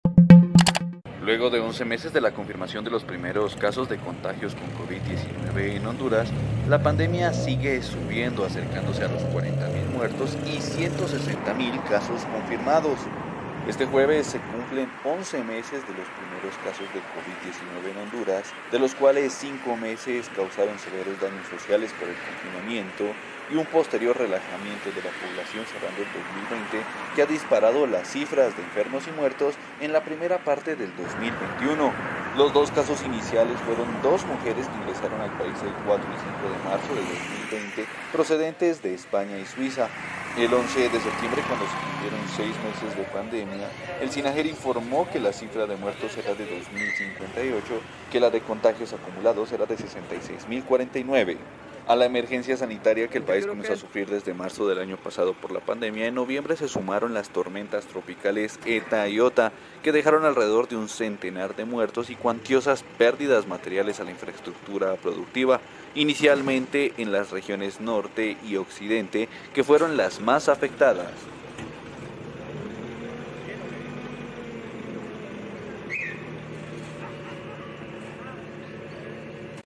Honduras se acerca a 4.000 muertos por covid-19 tras 11 meses de la pandemia
A 11 meses de que fueron confirmados los primeros dos casos de contagios con covid-19 en Honduras, la pandemia sigue en alza, acercándose a los 4.000 muertos y 160.000 contagios. Duración: 01:19